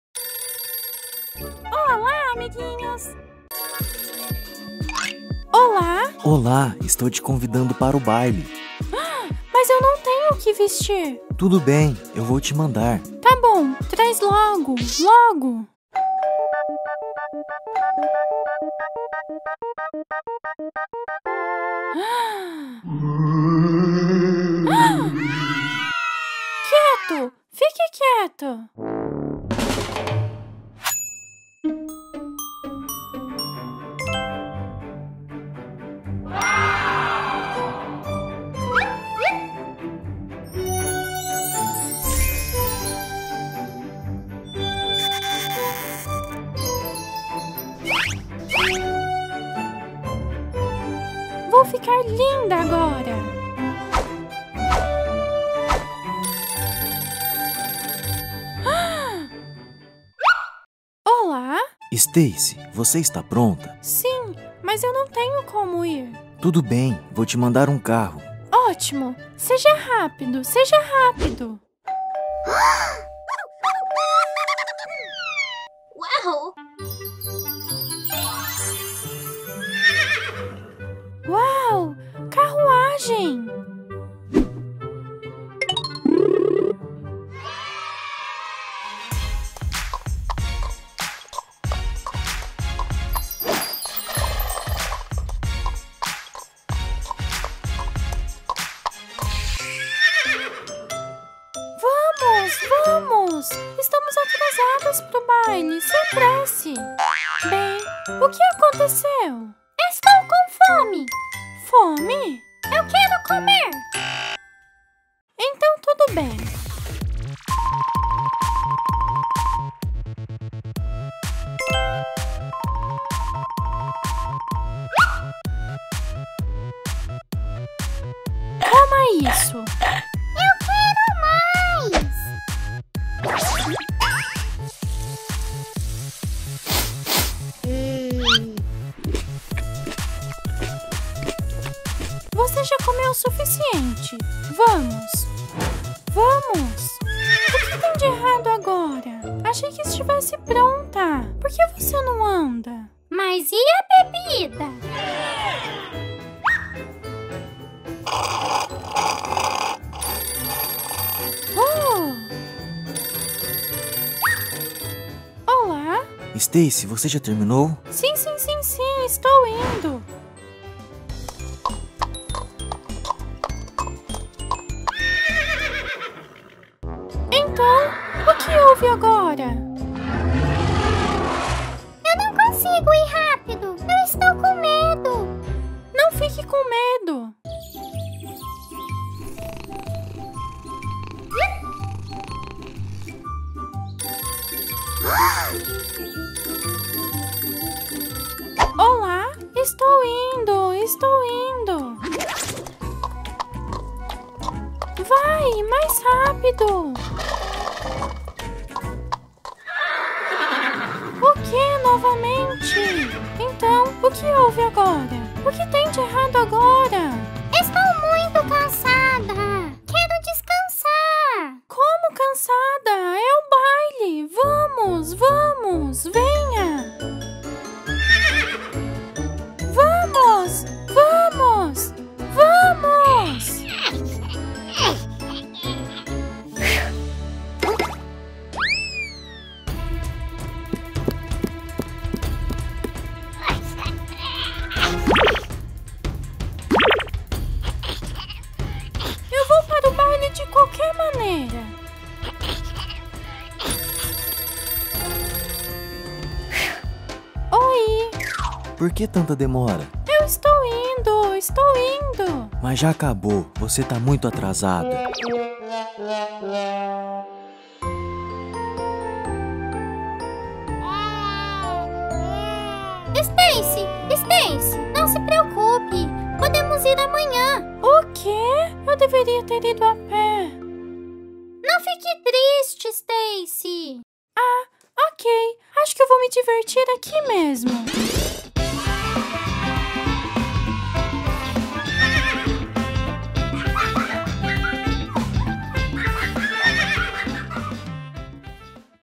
Stacy está indo para a bola
Stacy se veste e se apressa para a bola. Mas o cavalo dela não quer andar.